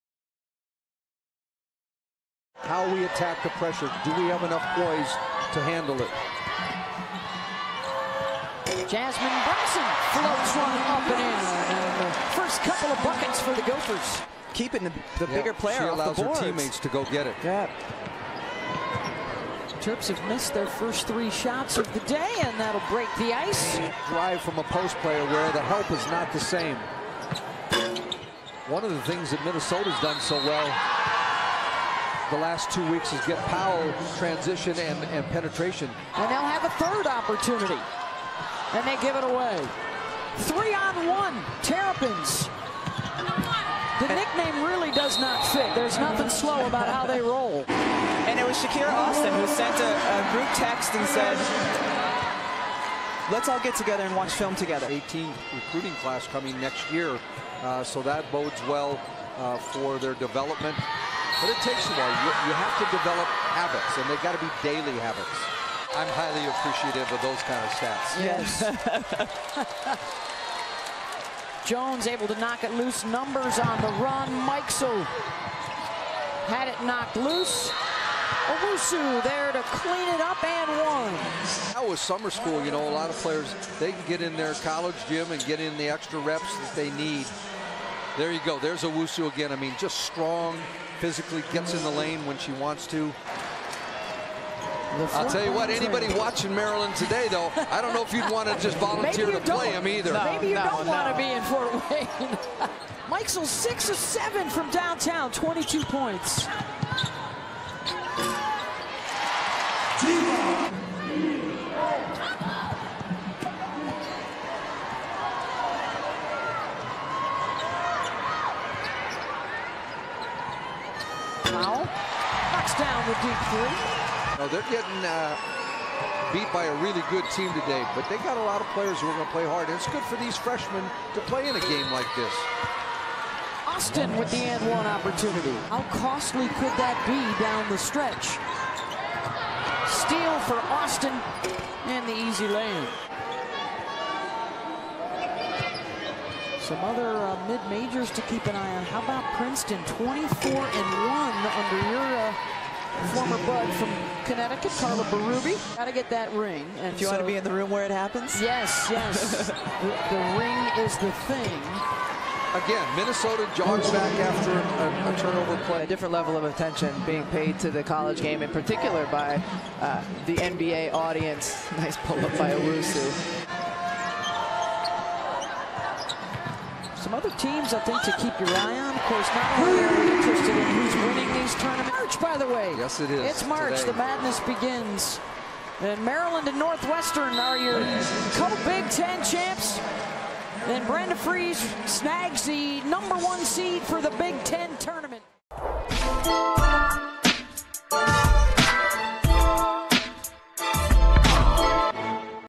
Highlights: Maryland at Minnesota | B1G Women's Basketball | March 1, 2020
Maryland's Taylor Mikesell scored 22 points in a 99-44 win over Minnesota.